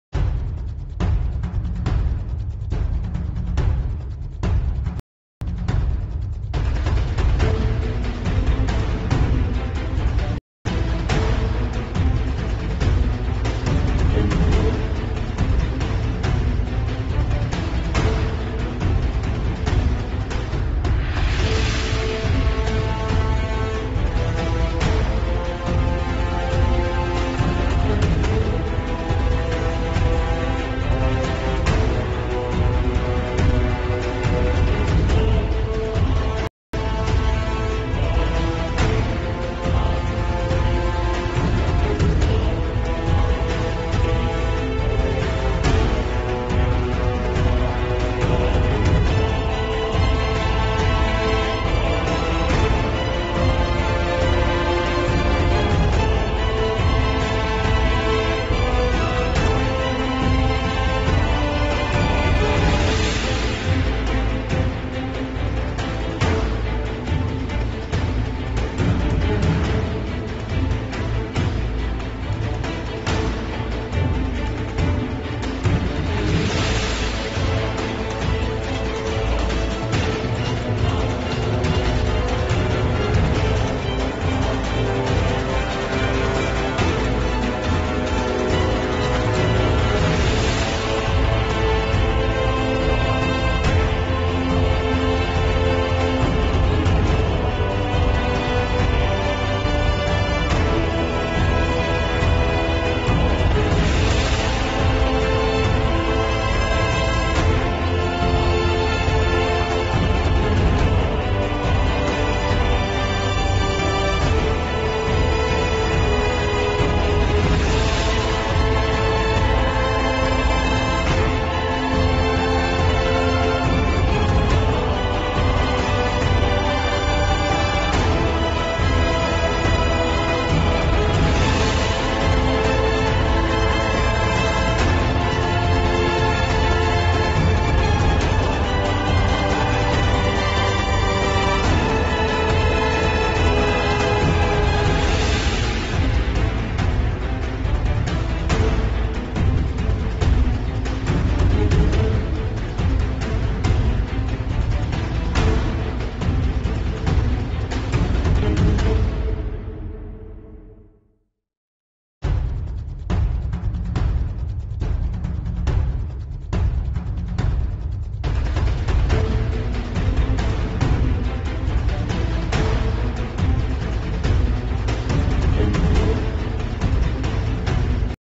How Many Places Do You See??? Kansas City Missouri From The Sky @ WWII Museum
A little sight seeing with my drone in Kansas City, Missouri.